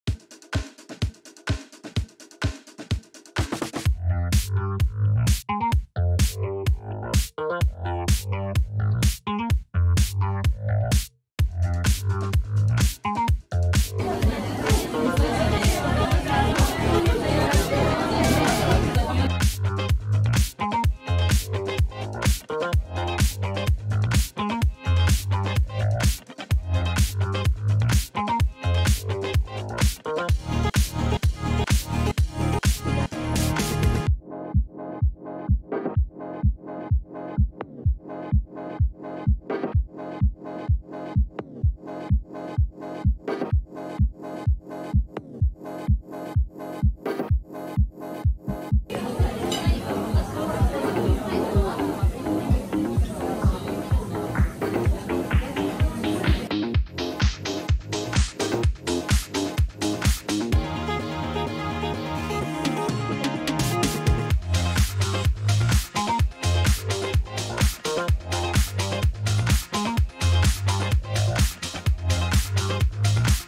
【名古屋グルメ】オススメフレンチ！シェ•トト！本当に美味しいです。
名古屋のオススメグルメを紹介します！